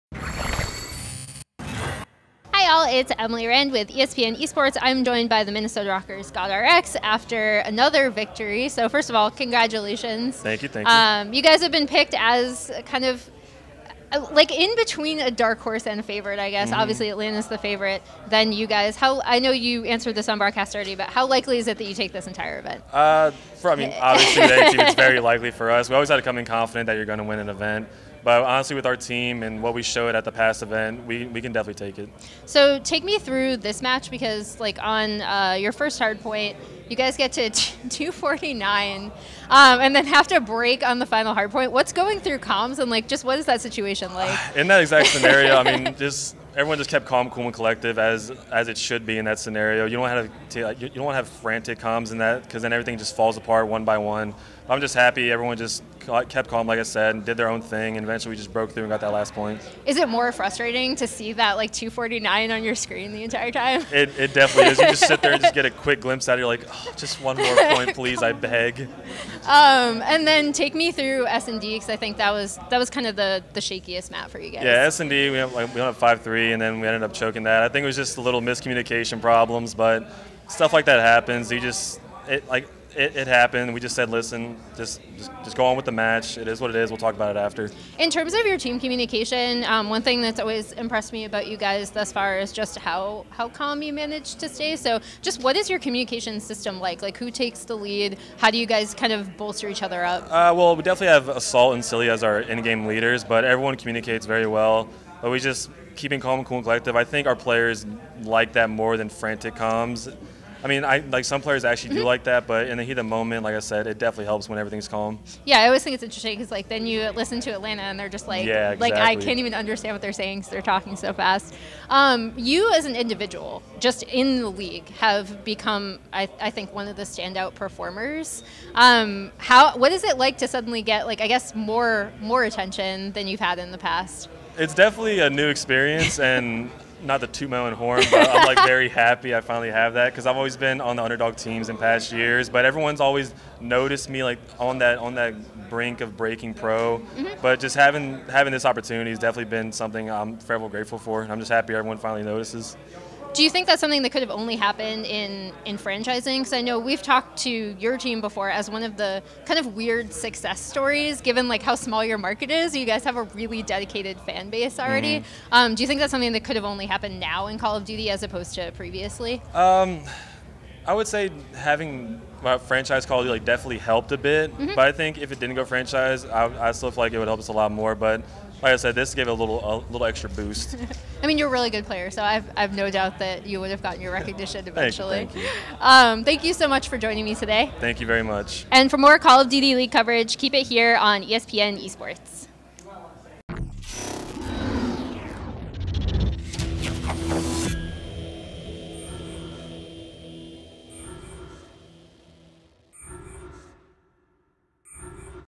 GodRX is glad to finally receive the attention he deserves | ESPN Esports
Adam "GodRx" Brown joins Emily Rand after his victory over the Seattle Surge at the Los Angeles home series weekend to talk about the match and his performance so far this season.